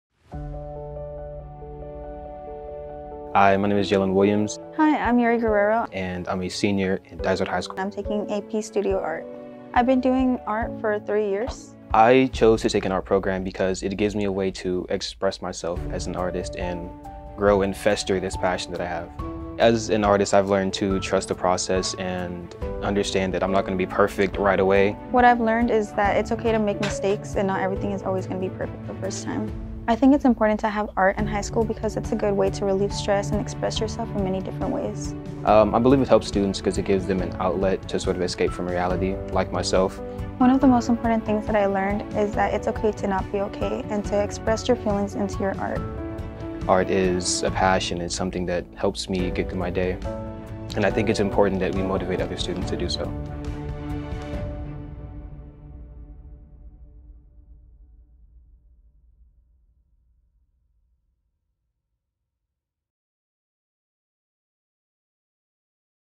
Arts in Education Week 2023 - Dysart High School Arts
Dysart Schools is celebrating Arts in Education Week by asking students to share what their art program means to them.  Students from Dysart High School art program talk about the importance of arts in high school education.